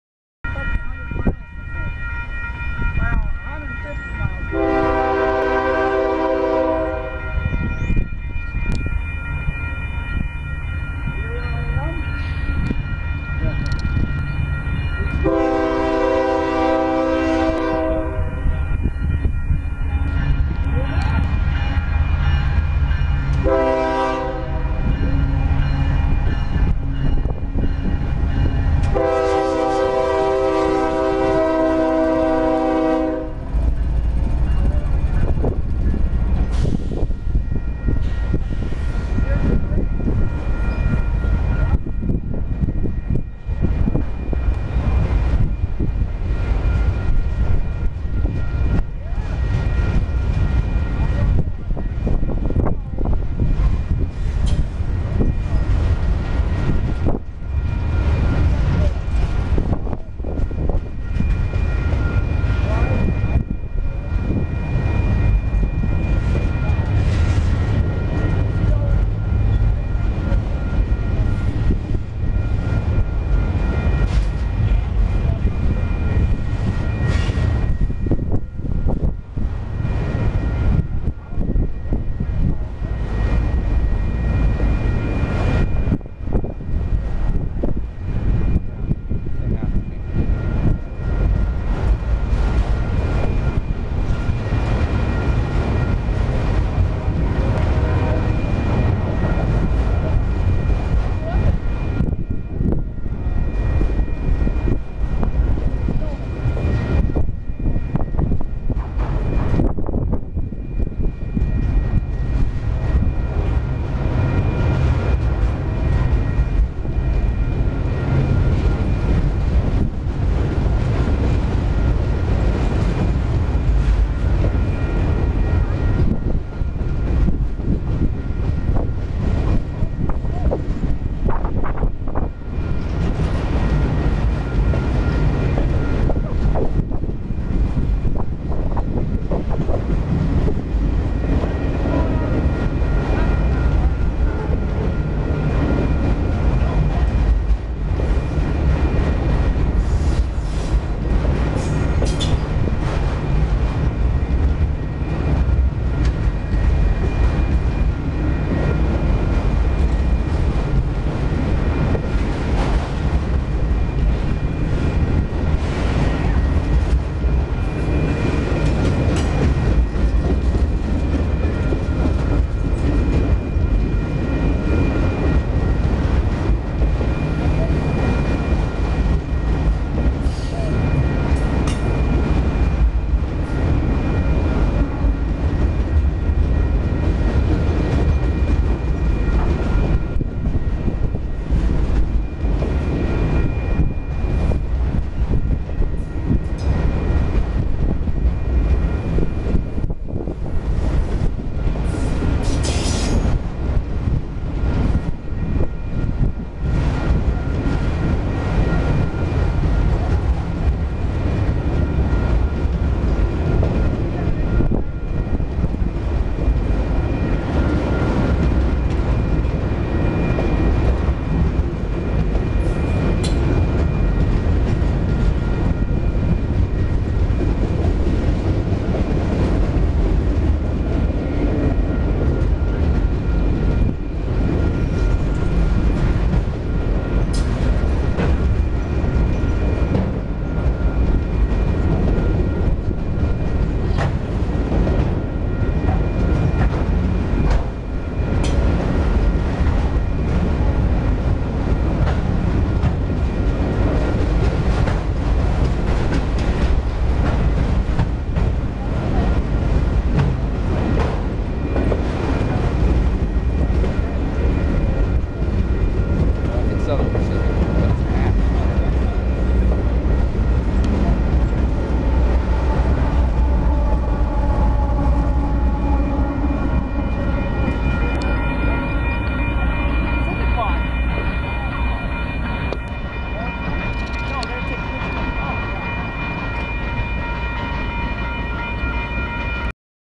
UP 7850 leads westbound autorack train
My boyfriend thought that the engine on the end, the Southern Pacific engine, was indeed special. I, like him, thought that the engine on the back was also quite neat.